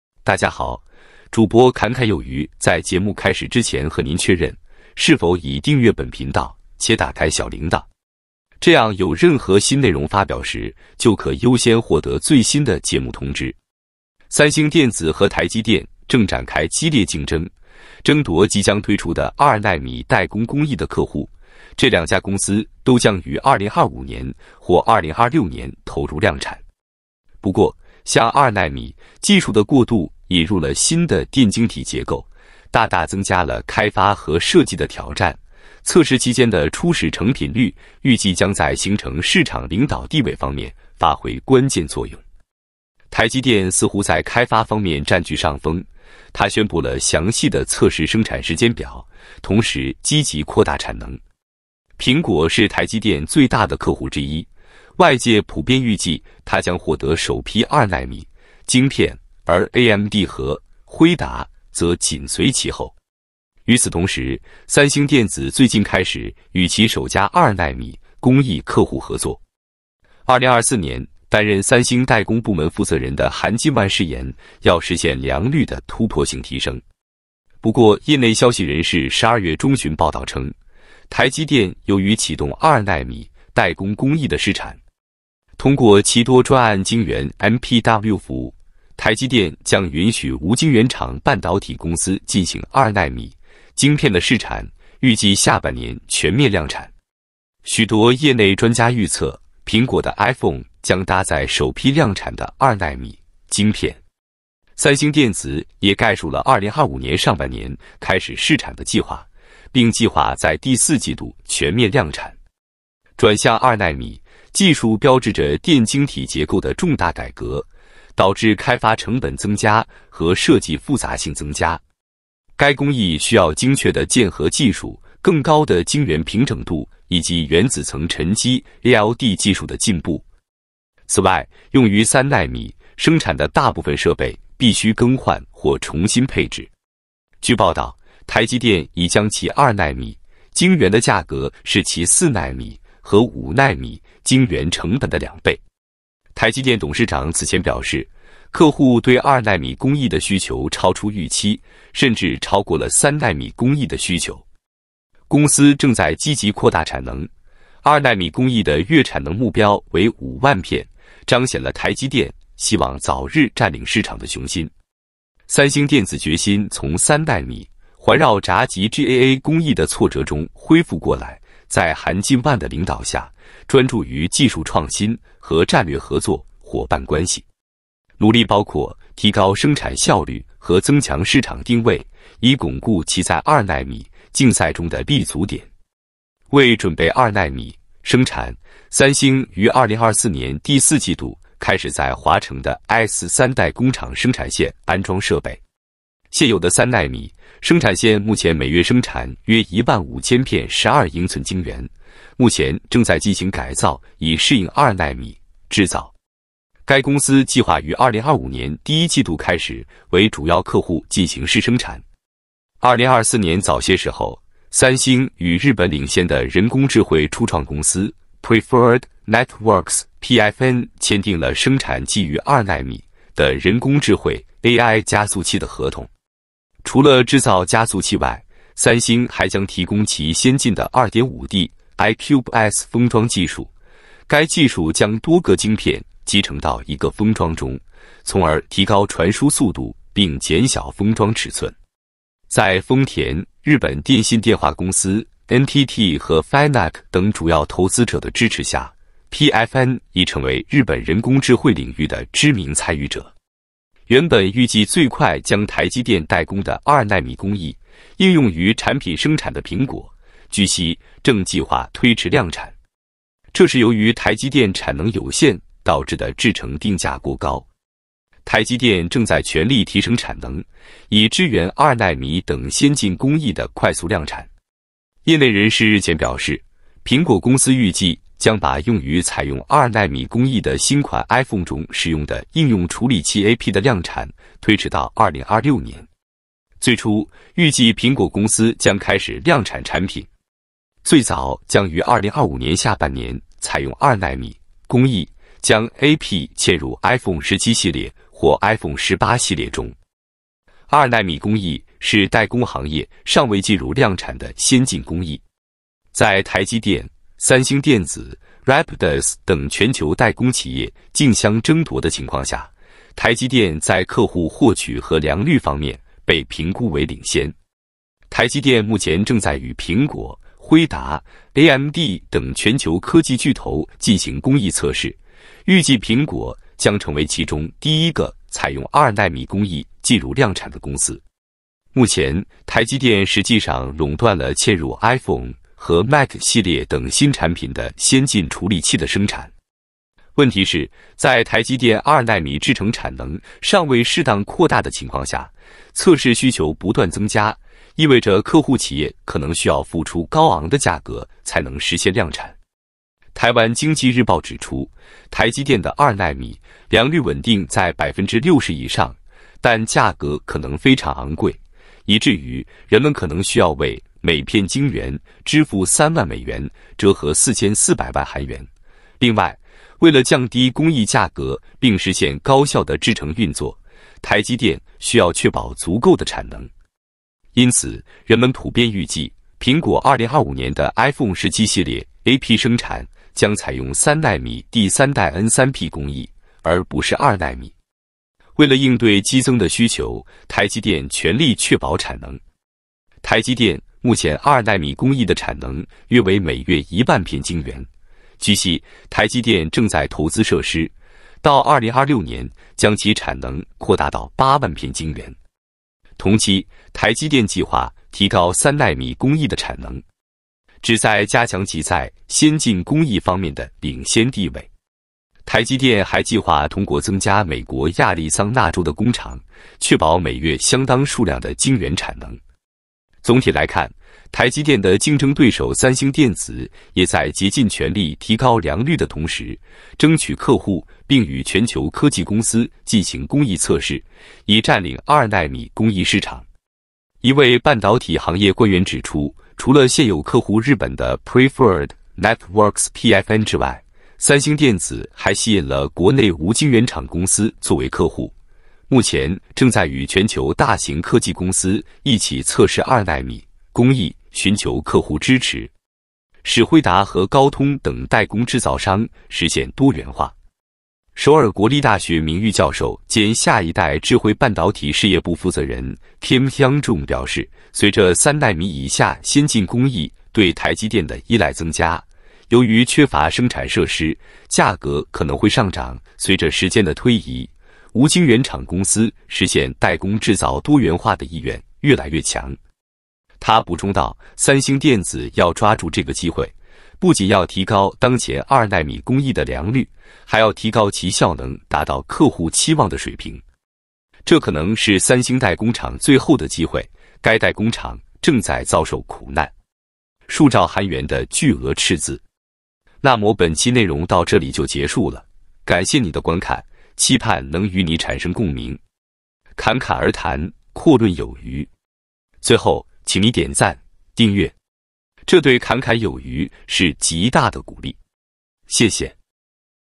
恐怖至極！台積電2nm巨貴報價震撼全球巨頭，利潤爆表讓這工藝難產的三星氣暈。為了應對激增的需求，正馬力全開全面確保產能。2奈米晶片難度遠超想象，需要精確的鍵合技術、原子層沈積ALD技術的進步。
業內消息人士 12 月中旬報道稱，台積電由於啓動 2nm 代工工藝的試產。通過其多專案晶圓 MPW 服務，台積電將允許無晶圓廠半導體公司進行 2nm 晶片的試產，預計下半年全面量產。三星電子決心從 3nm 環繞閘極 GAA 工藝的挫折中恢復過來，在韓進萬的領導下，專注於技術創新和戰略合作夥伴關係。努力包括提高生產效率和增強市場定位，以鞏固其在 2nm 競賽中的立足點。為了應對激增的需求，台積電全力確保產能。台積電目前 2nm工藝的產能約為每月10000片晶圓，據悉，台積電正在投資設施，到 2026 年將其產能擴大到80000片晶圓。